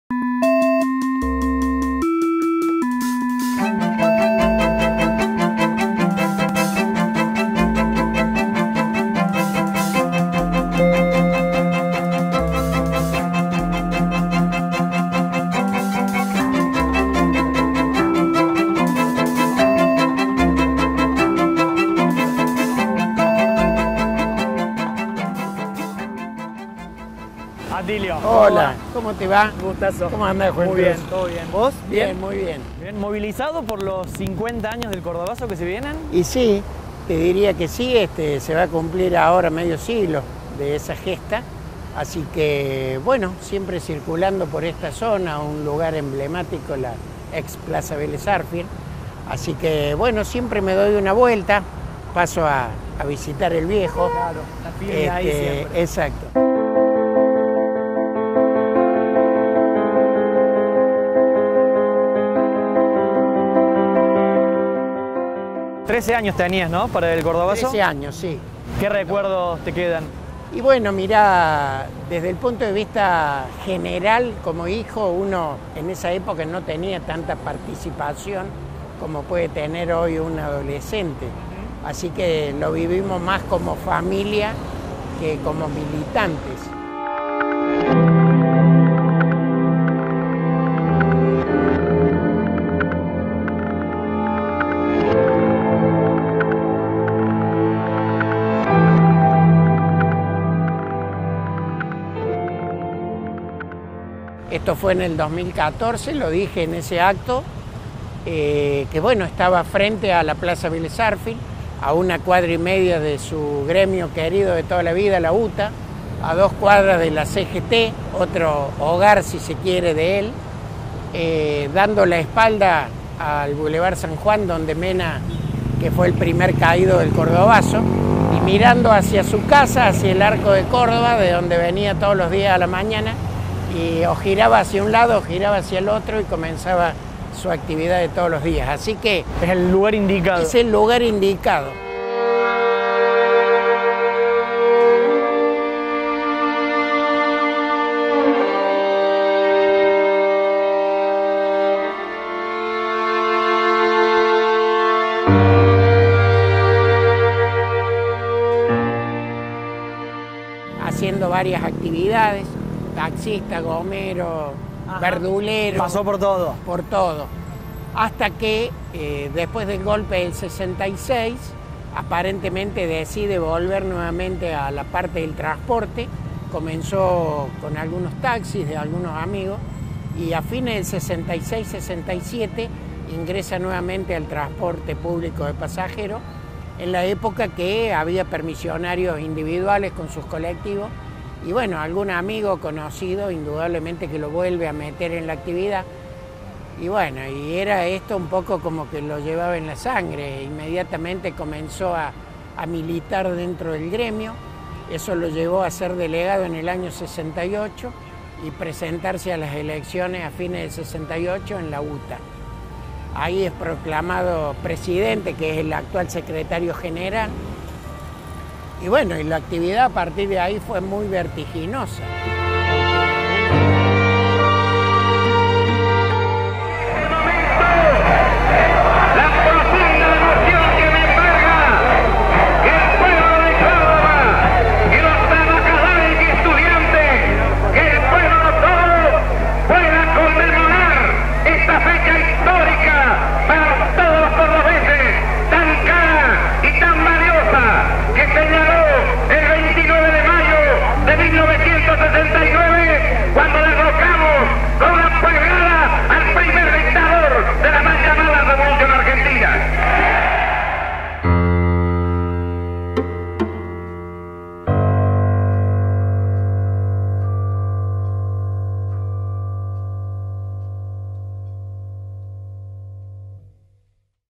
Cordobazo: Rebelión en la familia de Atilio
Atilio López (h) recuerda a su padre, el gran líder de la UTA y referente ineludible del Cordobazo.